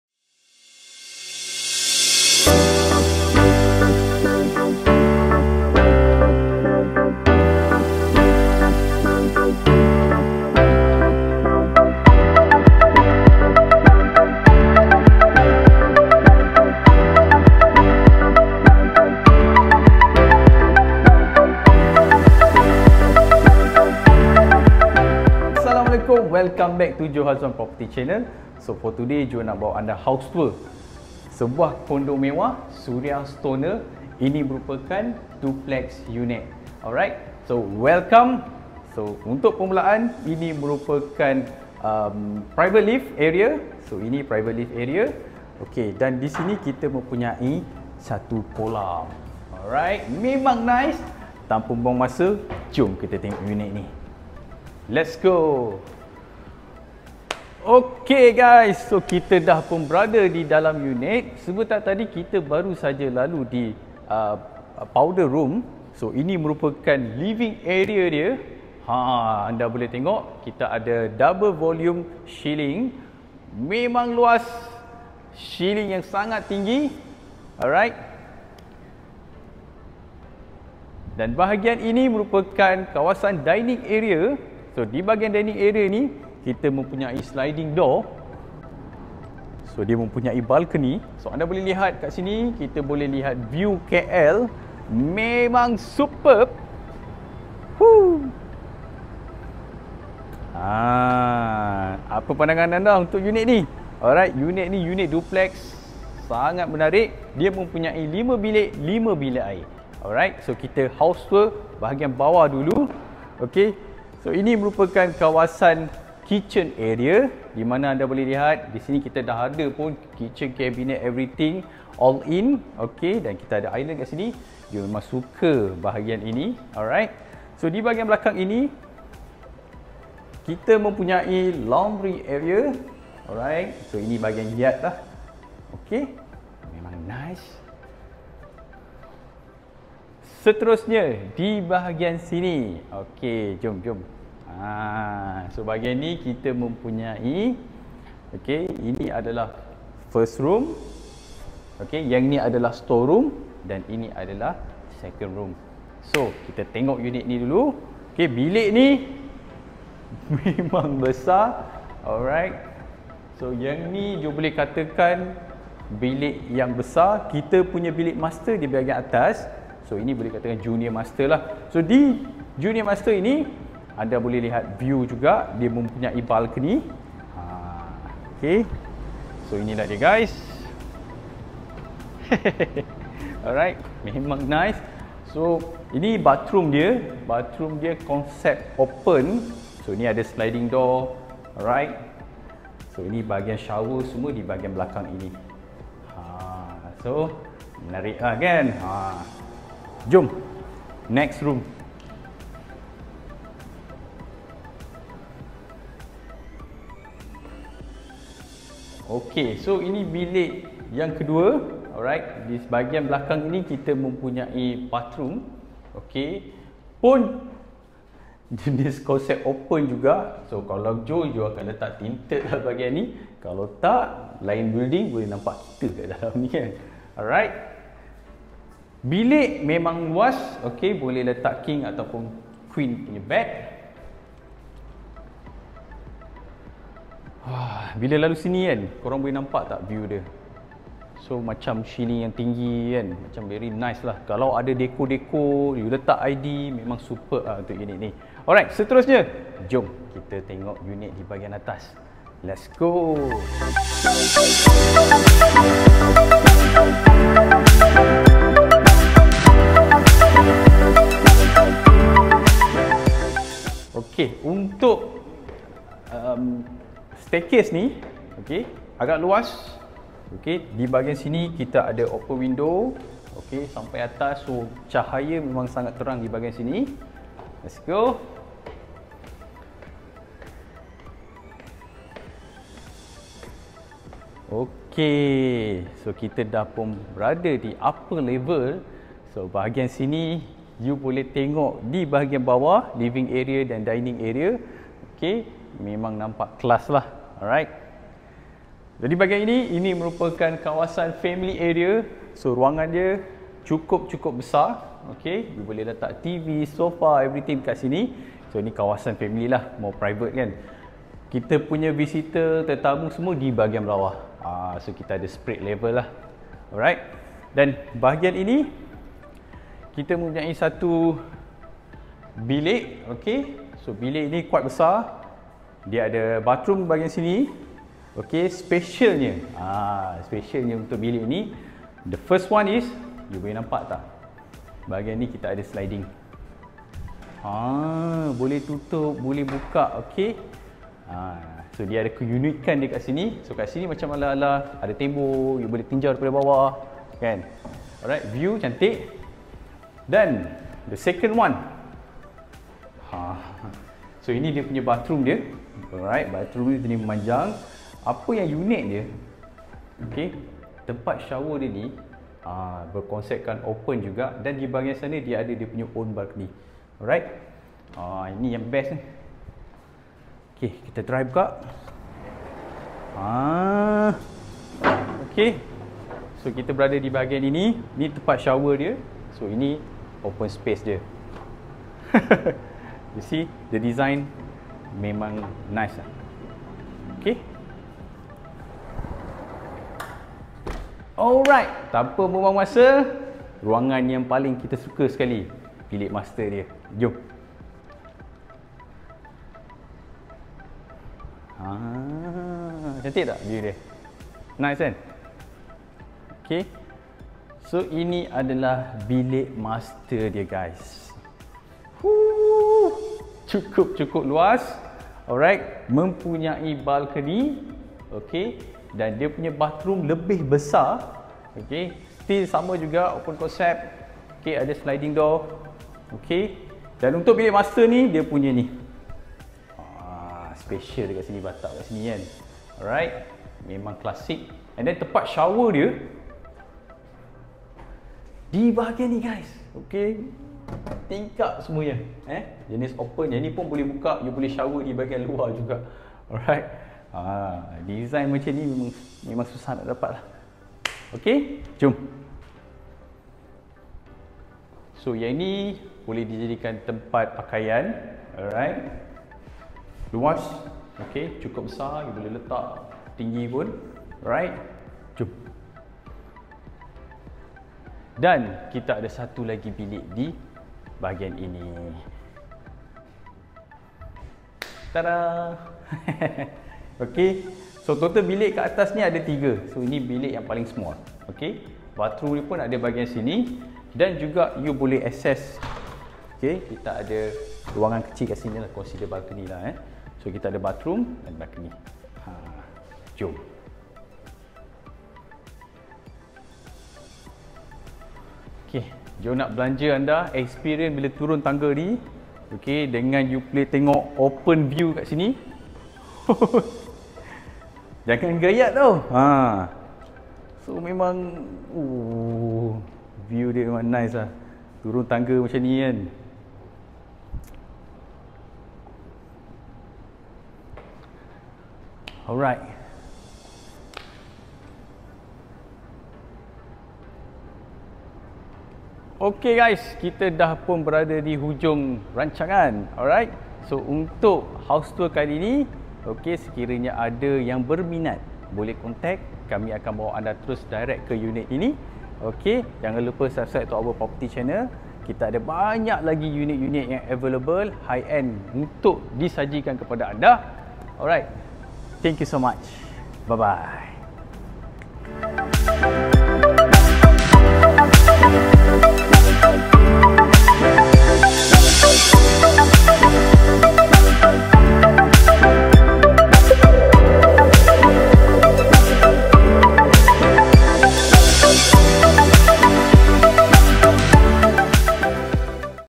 RM4,200,000 | SURIA STONOR SERVICED RESIDENCE @ KL | DUPLEX | 5412 SQFT | FREEHOLD | KLCC
[ Unfurnished] Suria Stonor Serviced Residence @ Kuala Lumpur For Sale !!!

Type : Duplex Serviced Residence
Build Up: 5412 sqft
No of Bedroom : 5+1
No of Bathroom : 5+2
Additional info:
- Freehold
- Actual unit as per photo
- Bare unit
- Facing KLCC
- Free SPA & loan legal fee
- Defect and warranty package (according to T&C)

Asking Sales Price: RM4,200,000 (Negotiable)